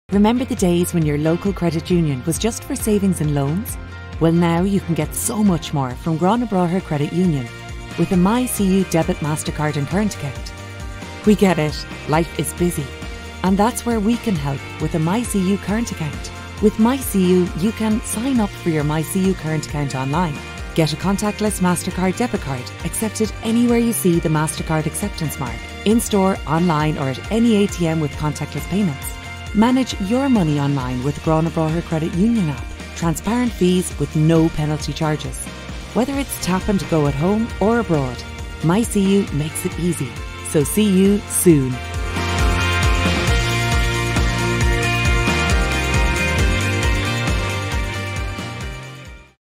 Gurranabraher Credit Union - MYCU Account
Promotional video PLAY created for Gurranabraher Credit Union as part of their over all social media strategy design by PLAY Creative.

PLAY x GCU
- Social Media Marketing Strategy
- Content Creation : Video, Photography, Graphics
- Social Media Page Management